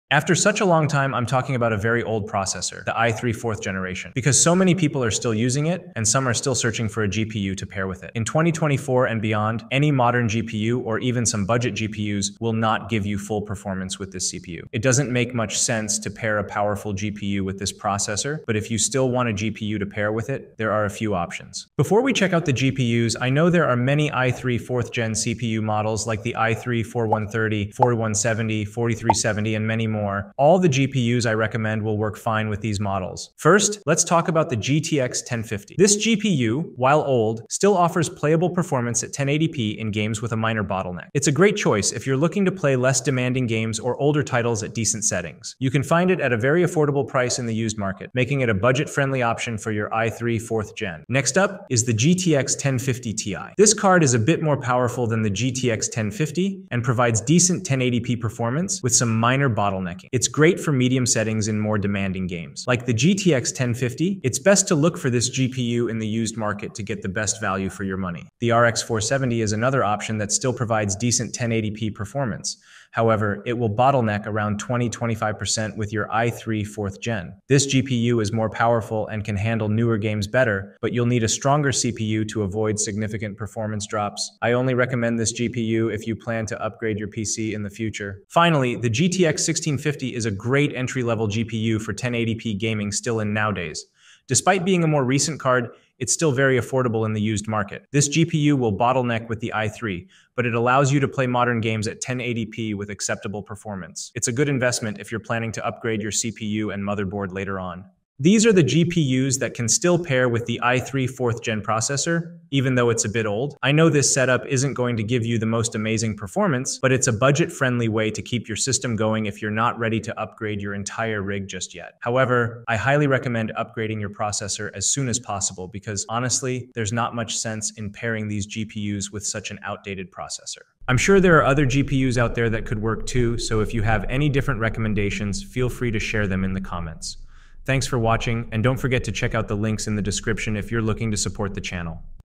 Best Graphics Card for i3 4th Generation
Best Graphics Card for i3 4th Generation in 2024 | Best GPU for i3 4th gen in 2024

Timelines
00:00 intro
00:32 Budget GPU for i3 4th gen
00:51 Best GPU for i3 4th gen
01:07 AMD GPU for i3 4th gen
01:29 Best 1080p GPU for i3 4th gen

Amazon India

Camera- Fujifilm X-T100
Mics-
Boya BY-MM1

Gamers Discussion Hub